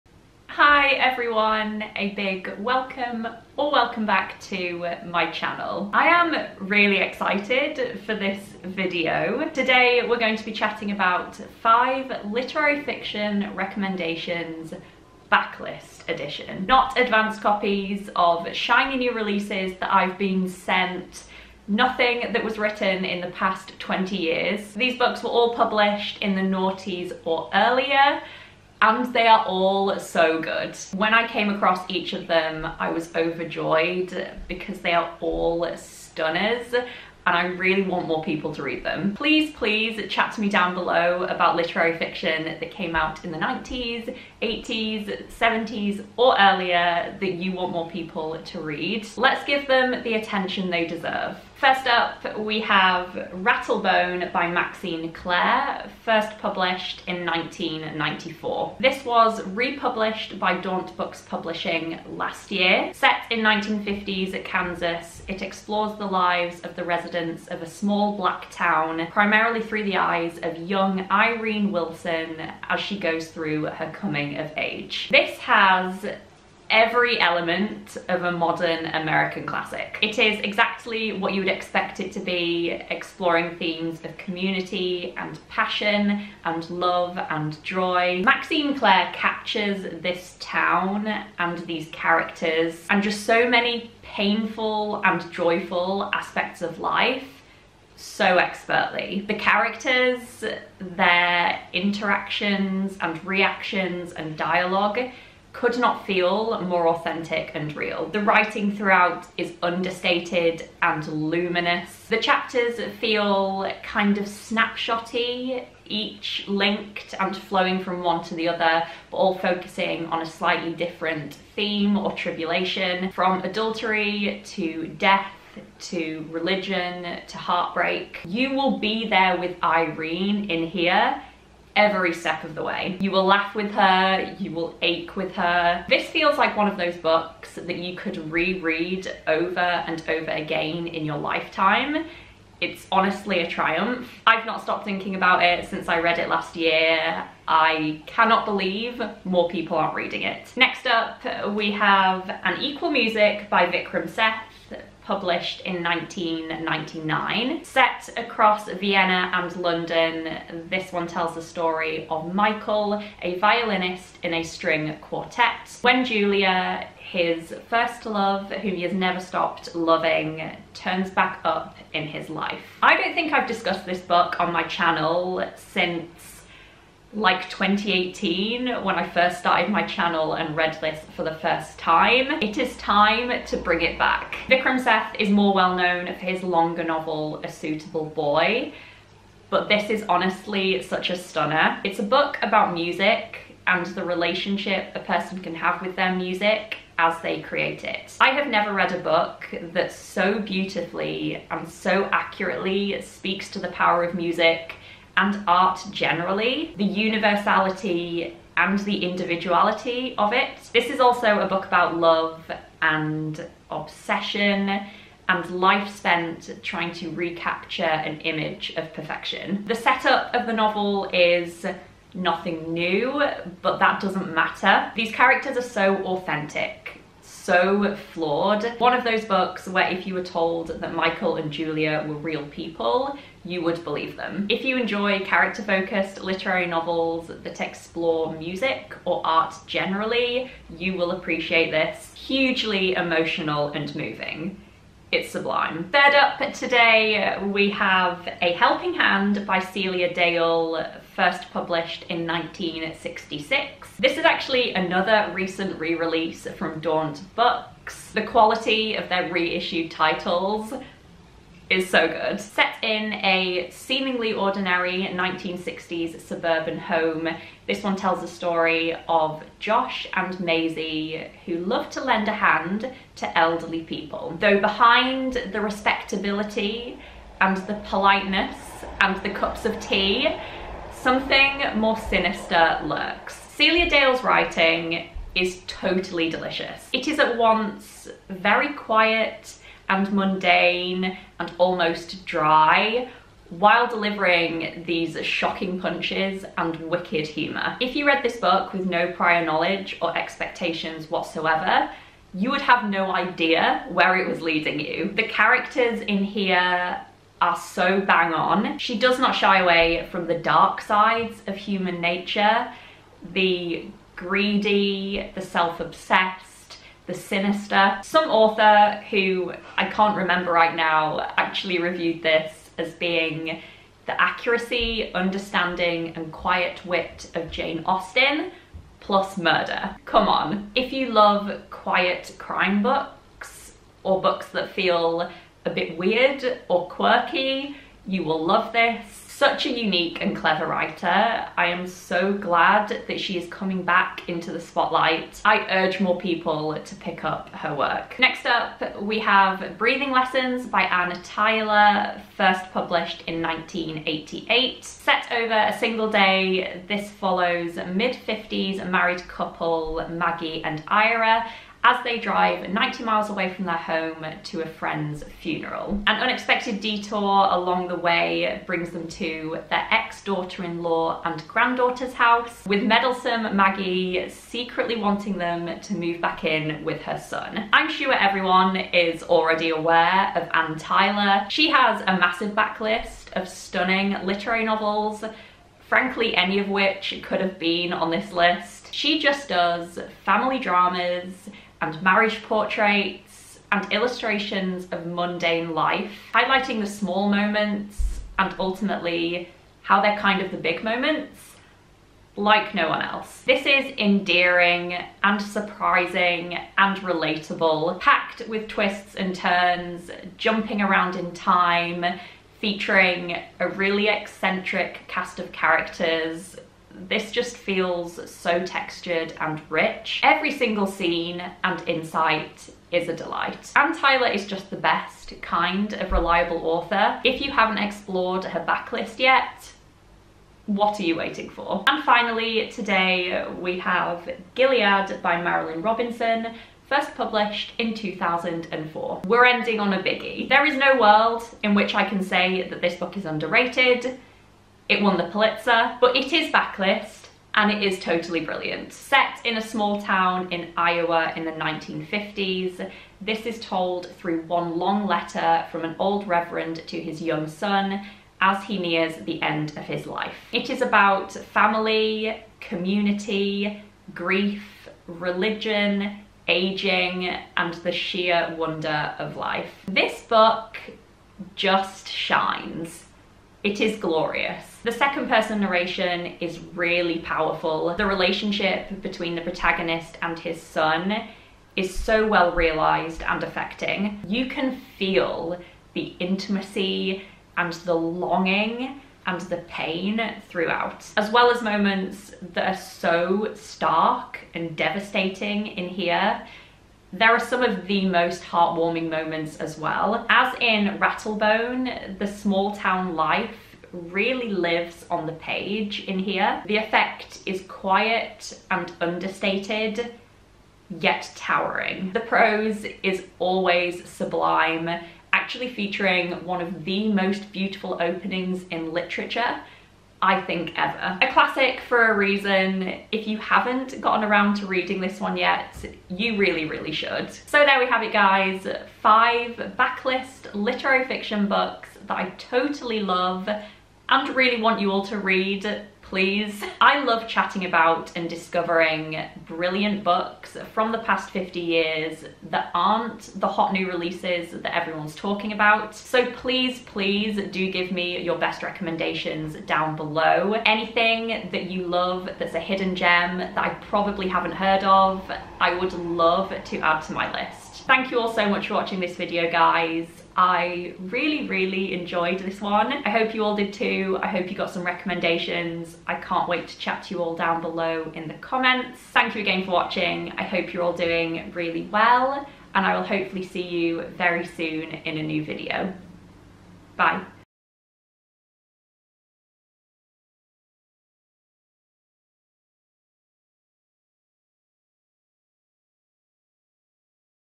5 books from the noughties and earlier you need to read 💙
Today we're bringing it backlist, chatting about 5 incredible novels from the noughties and earlier!

Not a shiny new release in sight 🤩

🛒 amazon wishlist